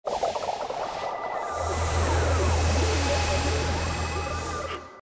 ТВ-3 - ID Космос (каша) 2016
Съёмки ID для Телеканала ТВ-3.
Режиссёр: Моисеев Дмитрий
Оператор: Федосеев Виктор
(апрель 2016)